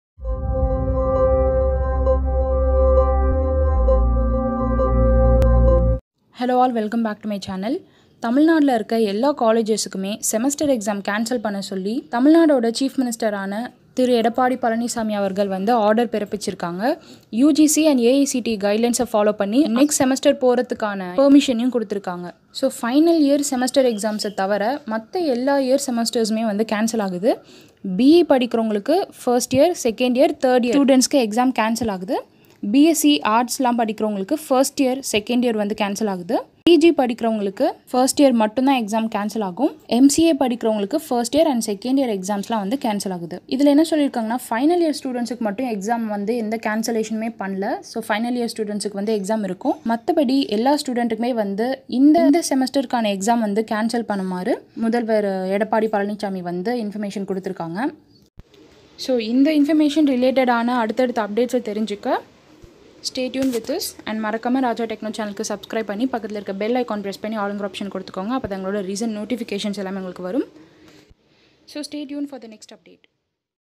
செமஸ்டர் தேர்வு ரத்து செய்யப்பட்டது | All college Semester Exam Cancelled
தமிழ்நாடு அனைத்து கல்லூரி செமஸ்டர் தேர்வு ரத்து செய்யப்பட்டது | All college Semester Exam Cancelled